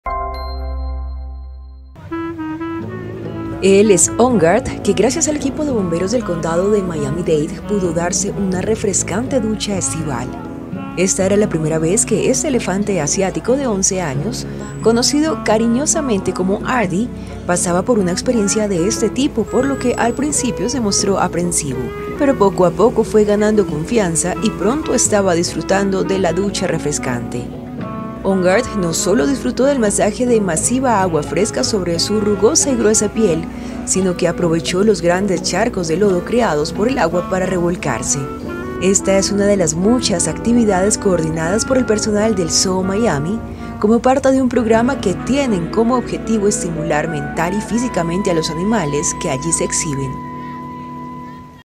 Elefante combate el calor estival de Florida bajo la manguera de los bomberos
El elefante "Ongard", del zoológico de Miami, pudo darse este miércoles una refrescante ducha estival, gracias a un equipo de bomberos del condado de Miami-Dade (Florida) que utilizó una boca de incendios del parque para conectar la manguera y bañar a placer al paquidermo.

Esta era la primera vez que este elefante asiático de 11 años, conocido cariñosamente como "Ardy", pasaba por una experiencia de este tipo, por lo que, al principio, "se mostró aprensivo".

IMÁGENES RON MAGILL/ZOO DE MIAMI

¡Visítenos!